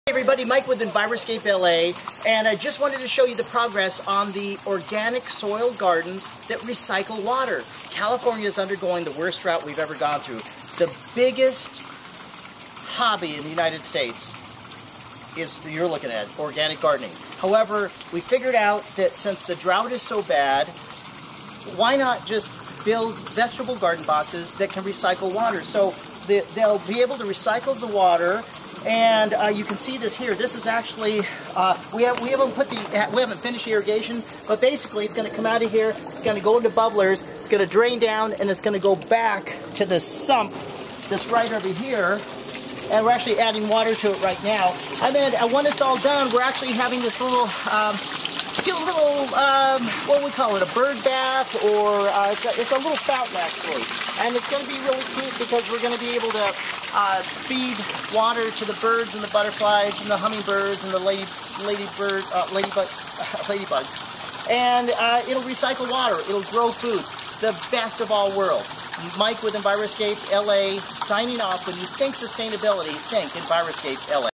Building a Soil Veggie Garden that Recycles Water
Enviroscape LA Founder Mike Garcia gives an update on the building of organic soil gardens that recycle water.  This shows some of the infrastructure before the finish of the project.